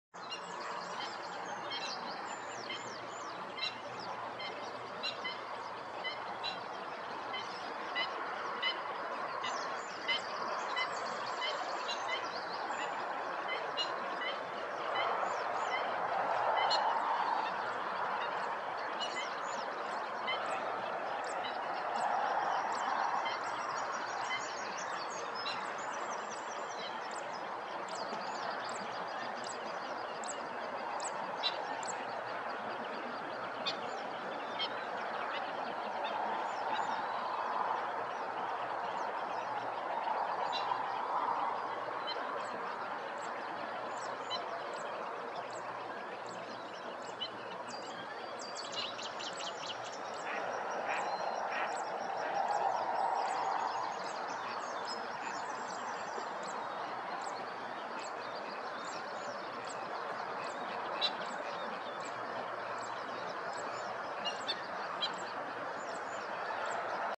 Correlimos zarapitín en las Marismas de Alday
Correlimos zarapitín (Calidris ferruginea) en plumaje nupcial en las Marismas de Alday, municipio de Camargo (Cantabria), el 29 de mayo de 2022.